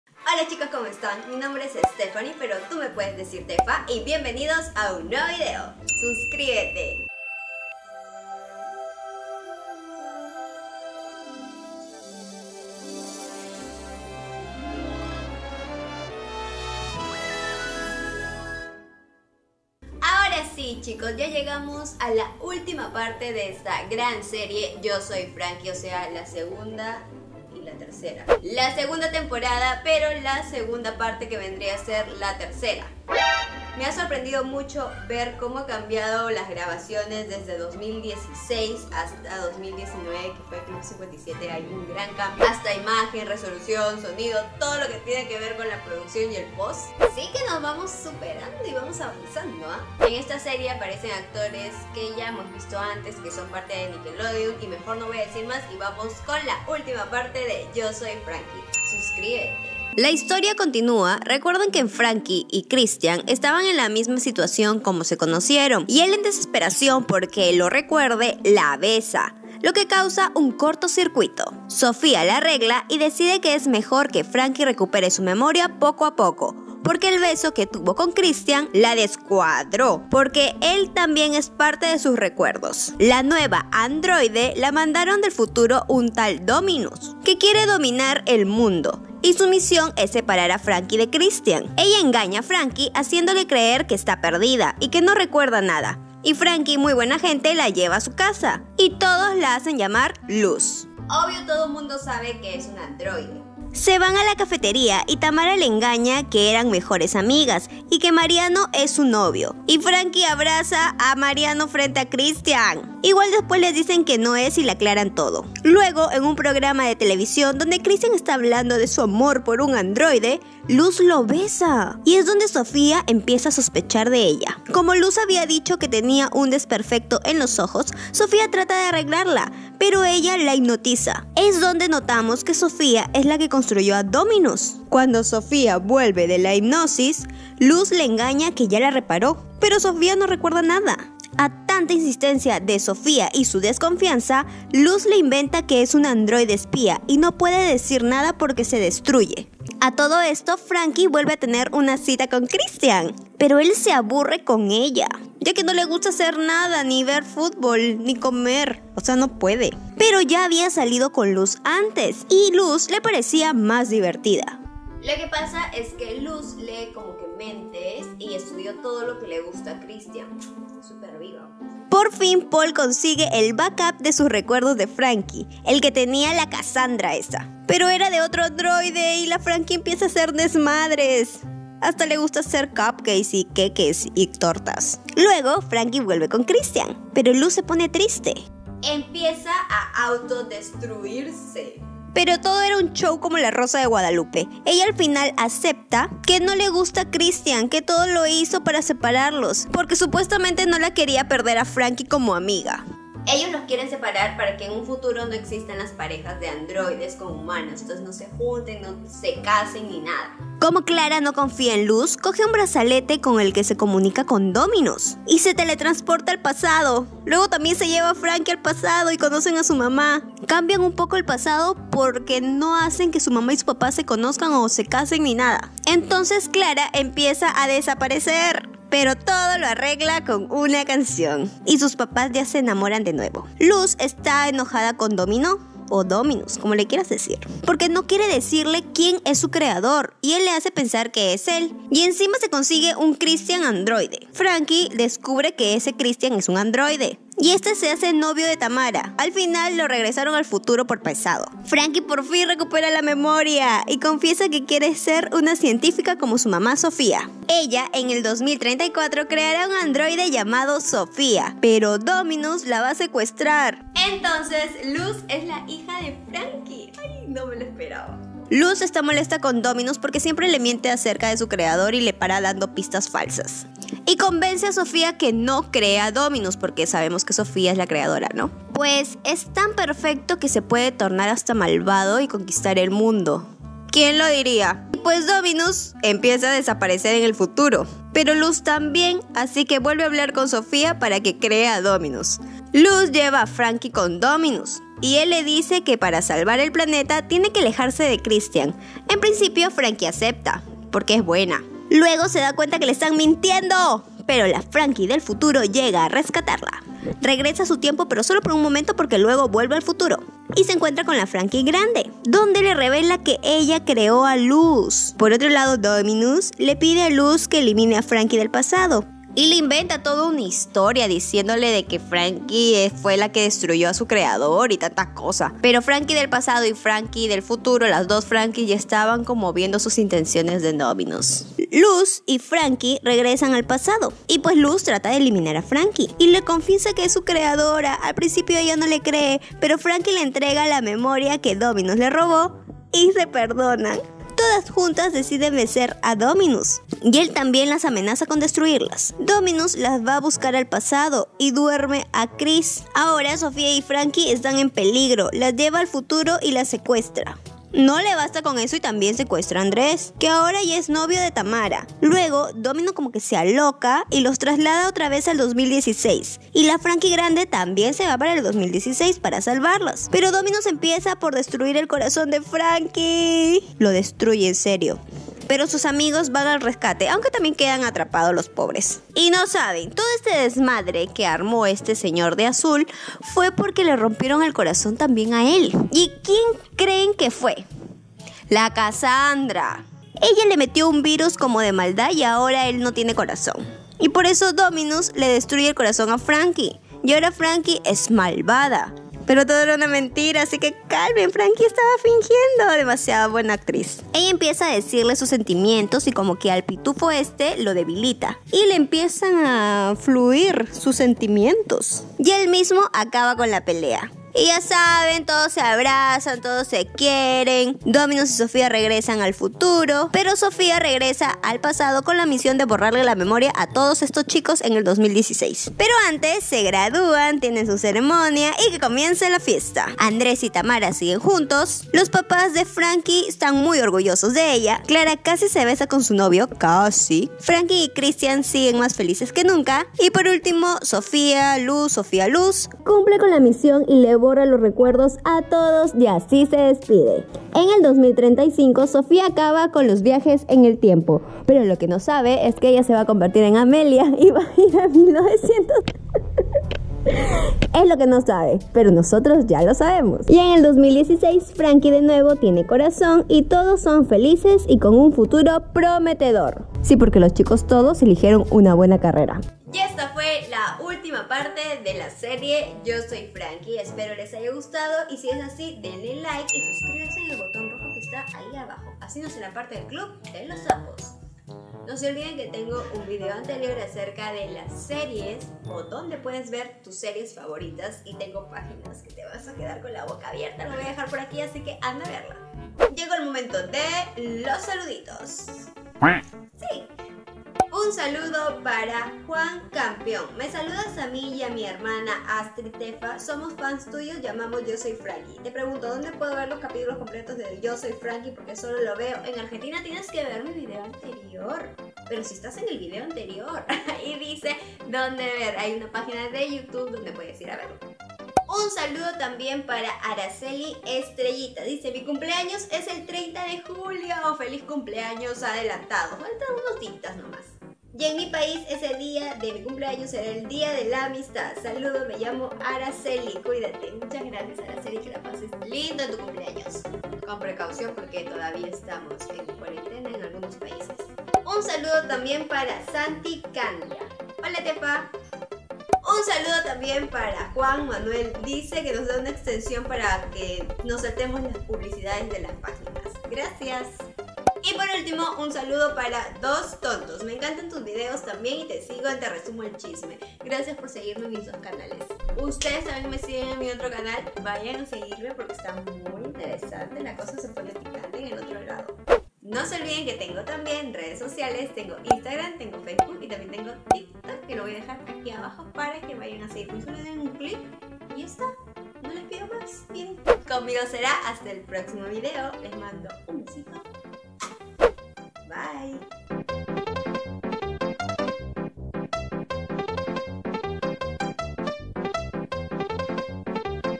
Resumen de Yo Soy Franky Tercera Temporada // Tefa Gonzalez
🧡Chicos y chicas aquí te resumo la serie YO SOY FRANKY SEGUNDA TEMPORADA

🌈 Encuéntrame en:

💛 TIKTOK  tefagonzalezc